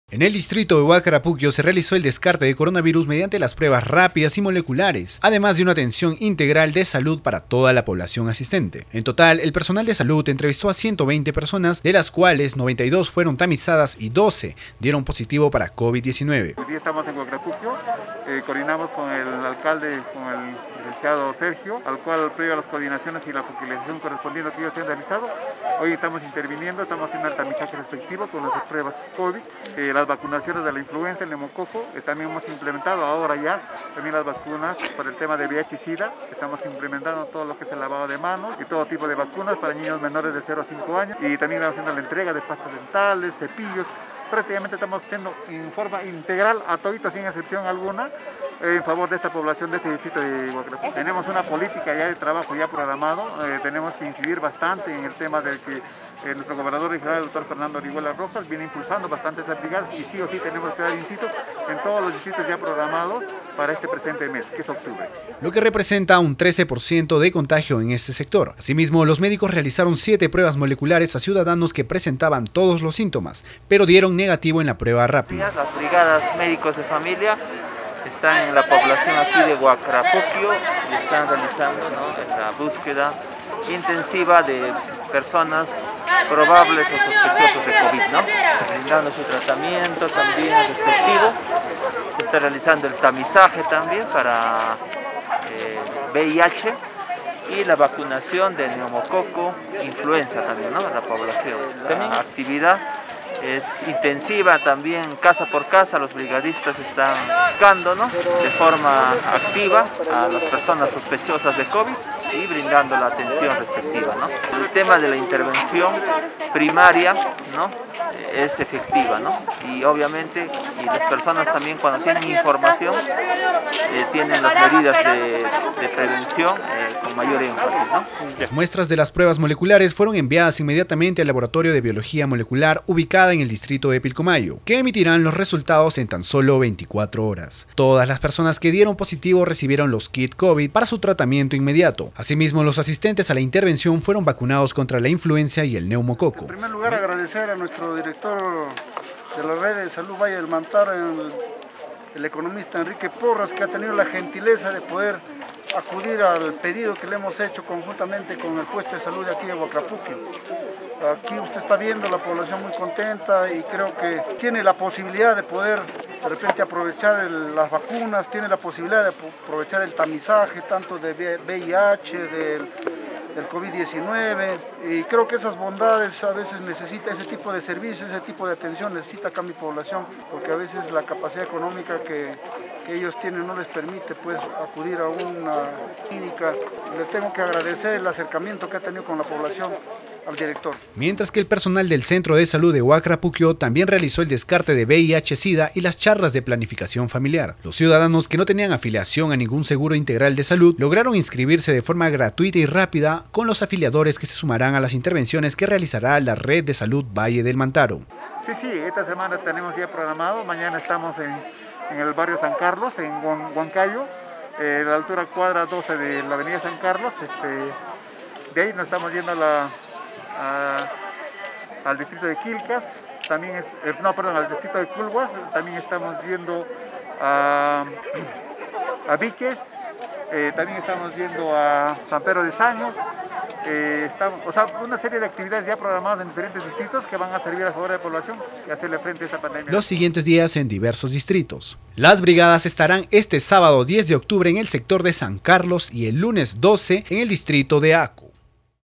HUANCAYO: De 92 personas, doce dan positivo a COVID | KDNA NOTICIAS
En el distrito de Huacrapuquio se realizó el descarte de coronavirus mediante las pruebas rápidas y las pruebas moleculares. Además de una atención integral de salud para toda la población asistente.
En total el personal de salud entrevistó a 120 personas, de las cuales 92 fueron tamizadas y 12 dieron positivo para COVID-19, lo que representa un 13% de contagio en este sector.